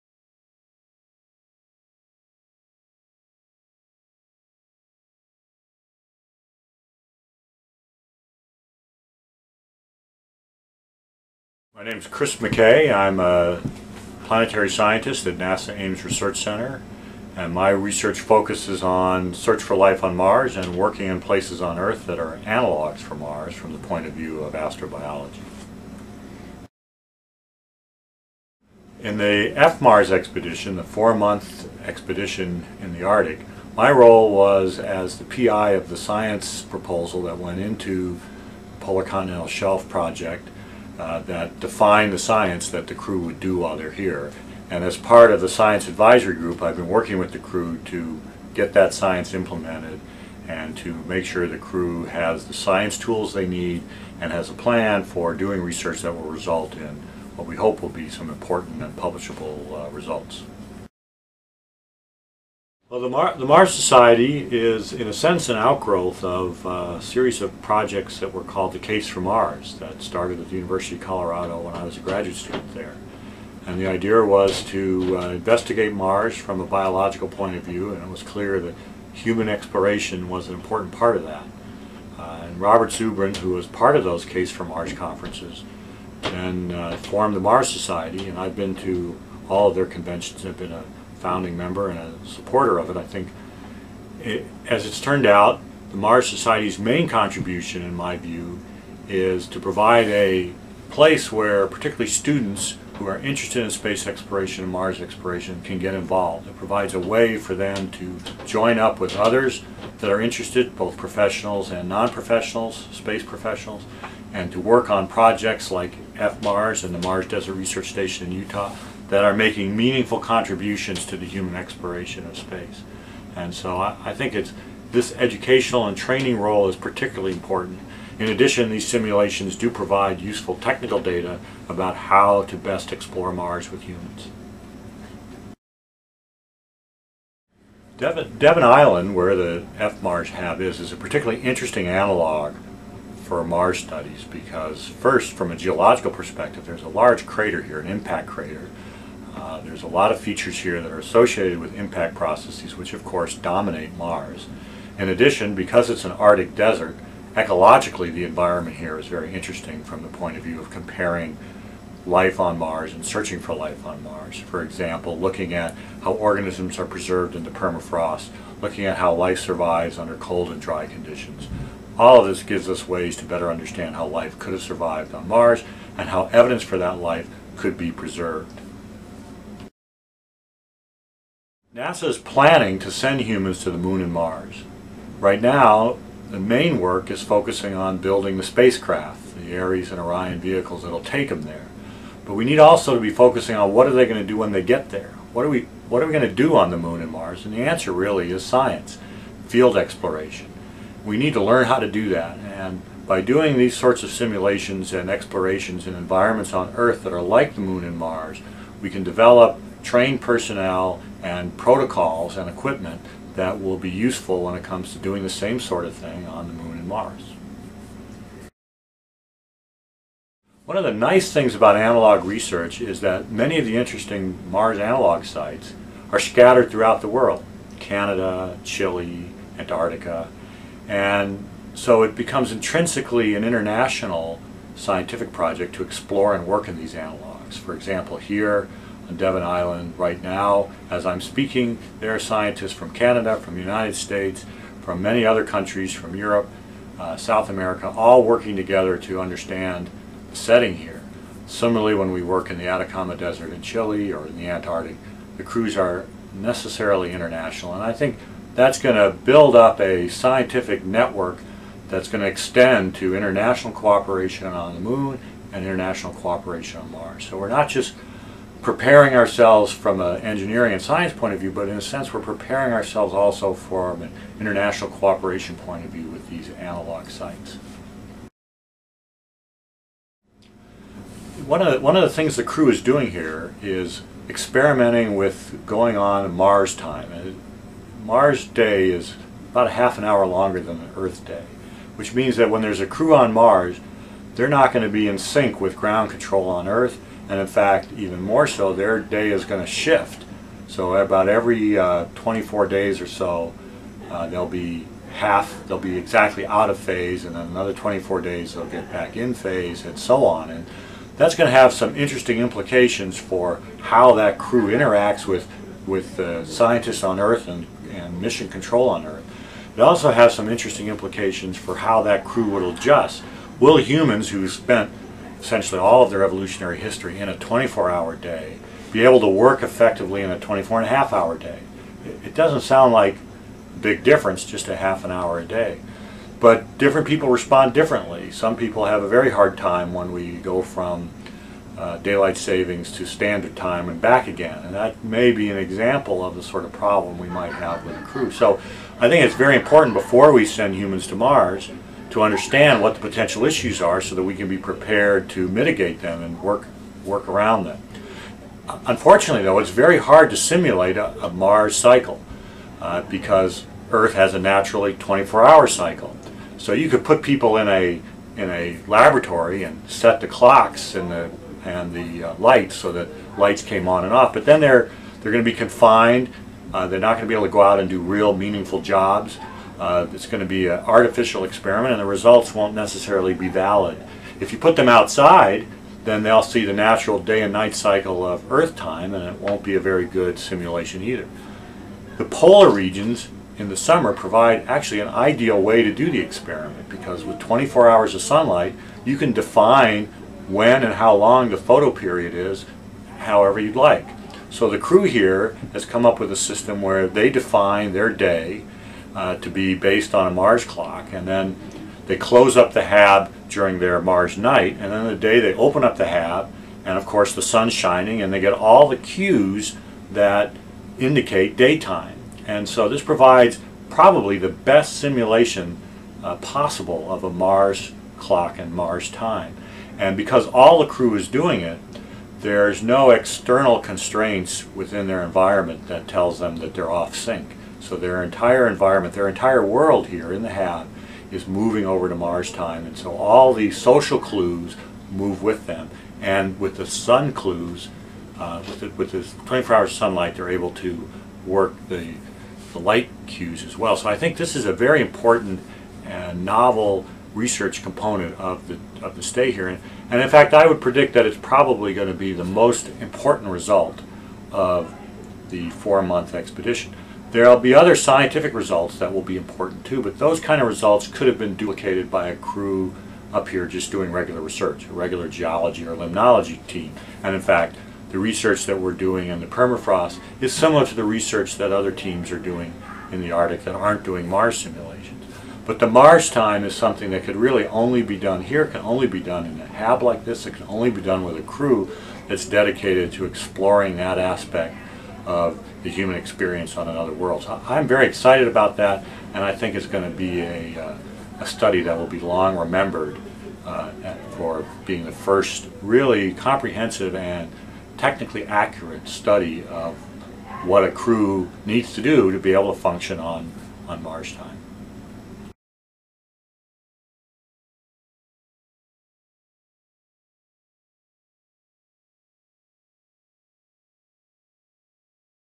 F-XI LDM Video Log Episode 19 - Chris McKay Interview Part 1
A two part interview with Dr. Chris McKay of NASA Ames Research Center!
It has begun! The F-XI LDM! Flashline Mars Arctic Research Station Crew 11 Long Duration Mission on Devon Island, Nunavut, Canada. A 4 month simulated Mars mission with a crew of 7, the first analogue Mars mission to ever be conducted for this length!